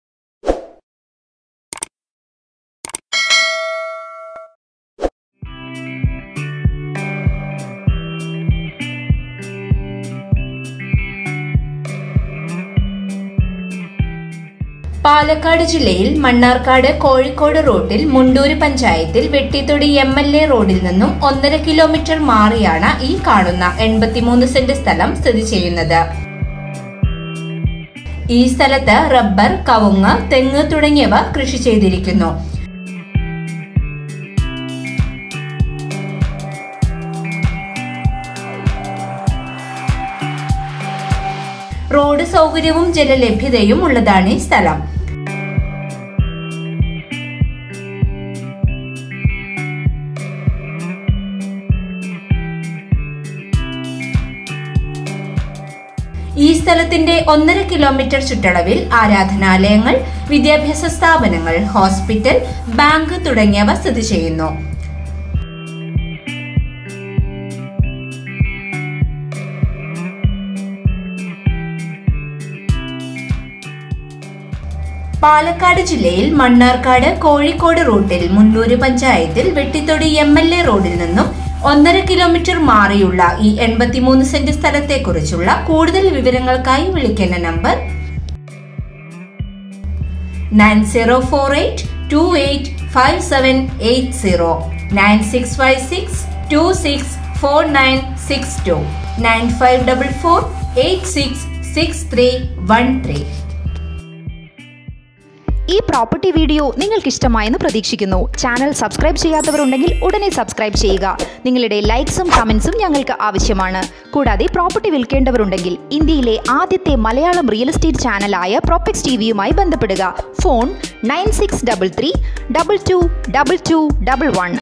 land for sale in palakkad| residential and commercial land in palakkad |Omana Pakd
Palakkad district.manarkad – kozhikodu route.mundoor panchayathu.1.5 km away from vettithittina  MLA road.83 cent land.rubber,areca,coconut are growing here.road and water facility.around 1.5km worships,schools,hospitals,bank.contact:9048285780,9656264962,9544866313